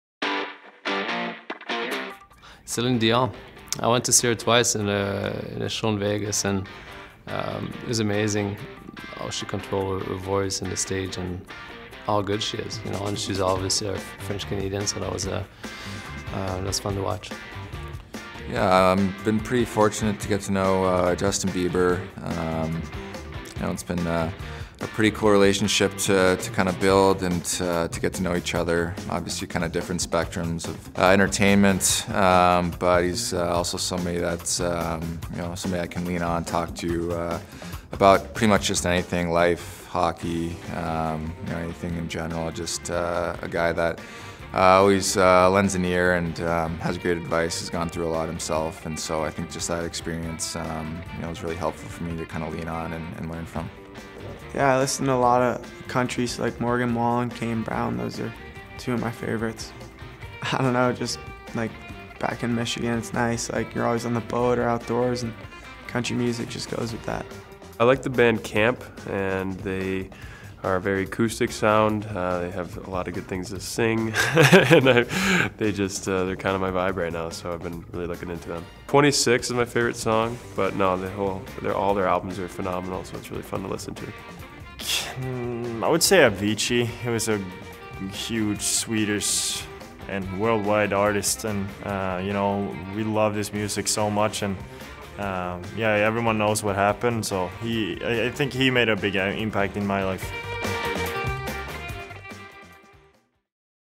Who Influences You? | Musical Acts
Marc-André Fleury’s love for this Canadian pop star is all coming back to him.

The Minnesota Wild netminder, along with a few of his fellow players, discuss which musical acts have made an impact on them.